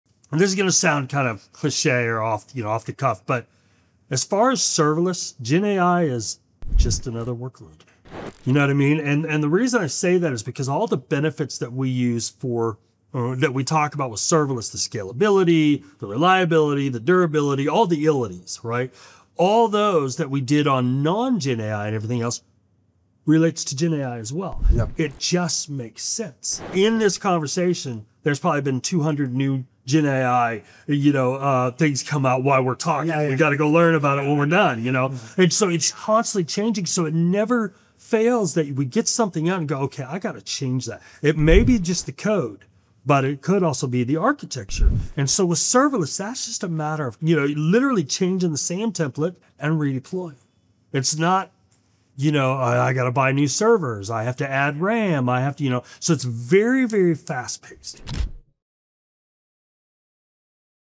The Gen AI truth nobody’s talking about!
Eric Johnson points out a perspective that flips the usual GenAI narrative, and challenges how we think about workloads in the cloud.

𝗪𝗮𝘁𝗰𝗵 𝘁𝗵𝗲 𝗿𝗲𝗲𝗹 𝘁𝗼 𝗳𝗶𝗻𝗱 𝗼𝘂𝘁 🎥